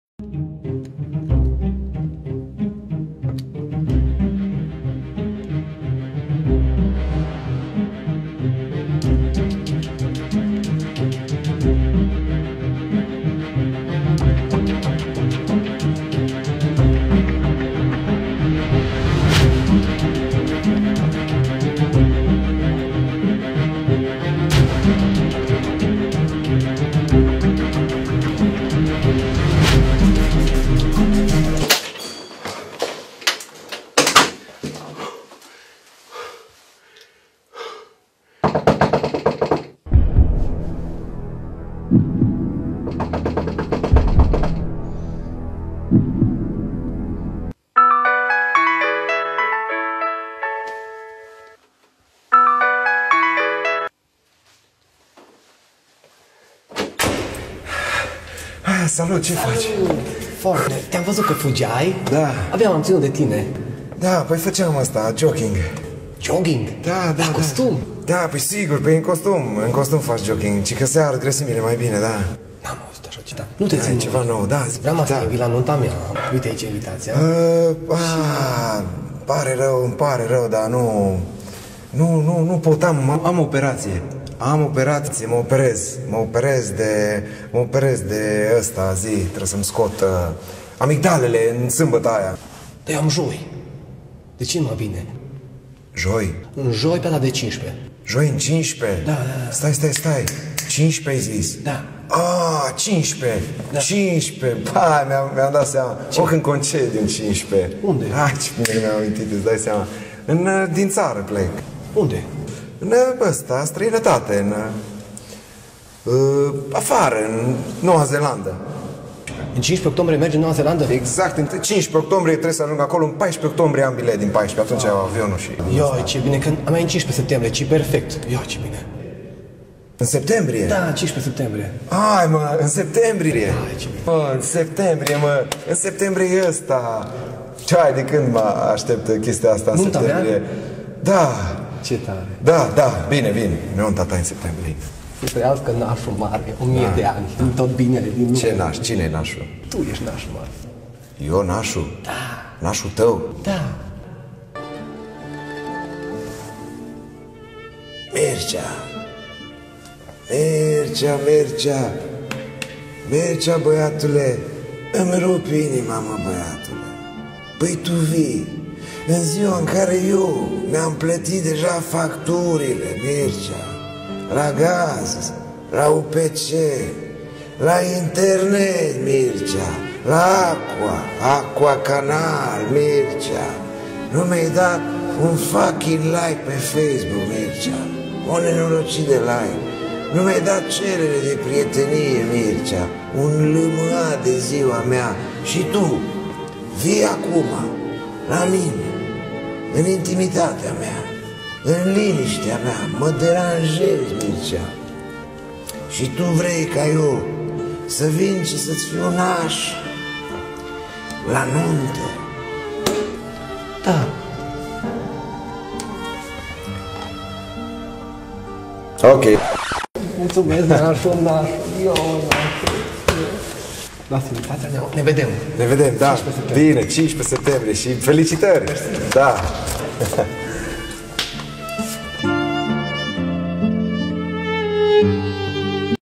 The Godfather, varianta românească (clip filmat în 2016)
Acest clip a fost postat acum 7 ani pe pagina mea de facebook. E și prima mea colaborare cu Adrian Cucu, primul actor care a acceptat să joace în clipurile noastre. Nu ne gândeam atunci că o să facem aceleași roluri in filmul Nuntă pe bani. Multumesc, Adrian Cucu!

Nuntă pe bani este în cinematografele din toată țara. Bilete în link-ul de mai jos